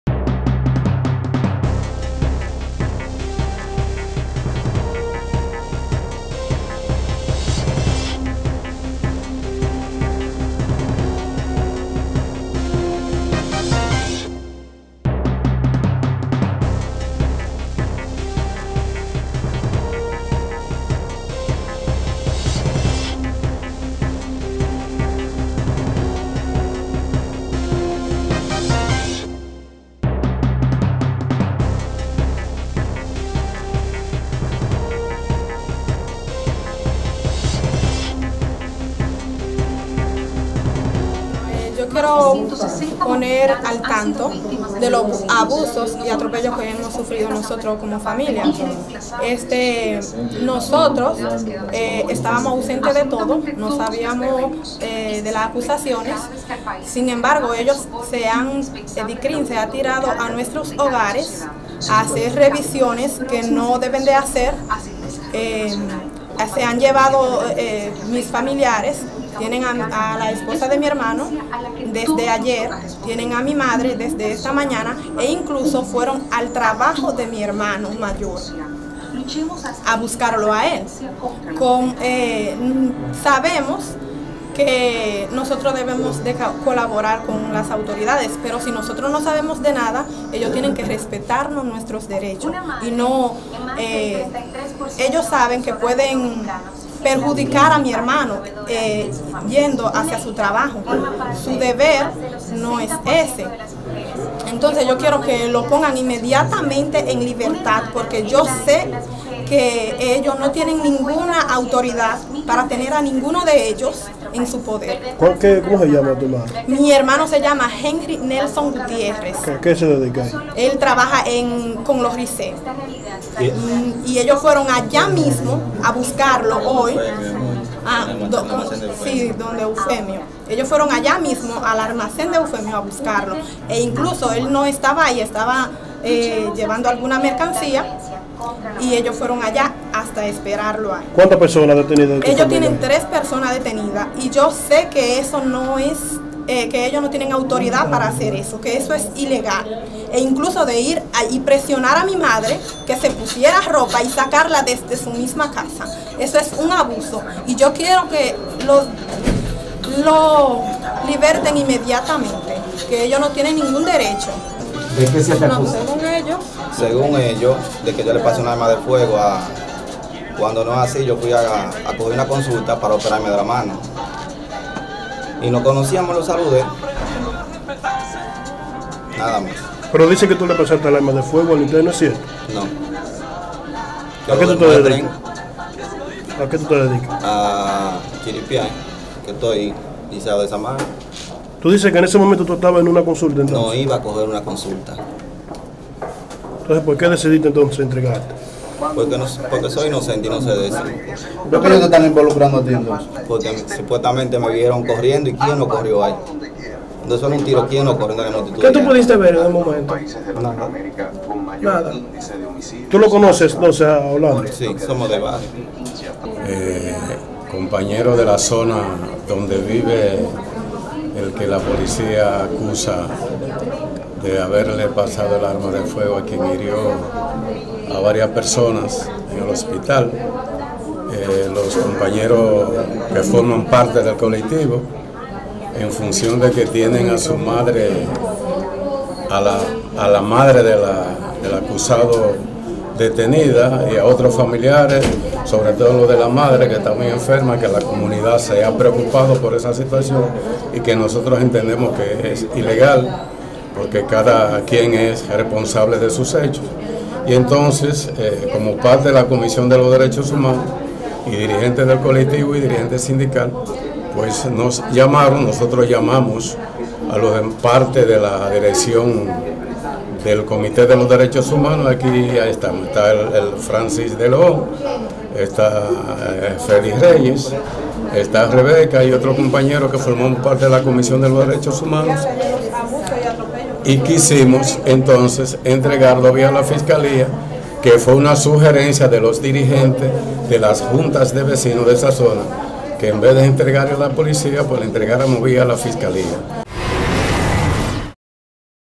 Se entrega “viva” acusado de suministrar arma a recluso intentó fugarse de hospital en SFM
San Francisco de Macorís -Se entregó ante la fiscalía del Distrito Judicial Duarte el joven José Aníbal Gutiérrez Severino alias Viva, quien era buscado por la Policía Nacional  acusado de complicidad en  intento de fuga de un recluso durante hecho registrado en esta ciudad.

Gutiérrez Severino se entregó a través de representante de la Comisión de Derechos Humanos y dirigente comunitario del sector Gregorio Luperón de esta localidad , quien pide se respeten los derechos del detenido .

Viva" es acusado de haber suministrado el arma de fuego a "Orlandito" cuando se disponía a escapar del área de consultas del San Vicente de Paúl el pasado miércoles.

Gutiérrez ,al ser cuestionado negó los hechos que le imputan , afirmó conoce al recluso pero que solo fue a tomar una cita para consultar posteriormente.

Recordamos en torno a este hecho que  Padilla cuando intentaba escapar hirió de bala a un agente de Vigilancia y Tratamiento Penitenciario (VTP) identificado como Aquino, también a Jan Carlos Rodríguez Vásquez, Manuel Díaz Rojas y Esteban Díaz Rojas, que se encontraban en la sala de consultas  del referido centro de salud.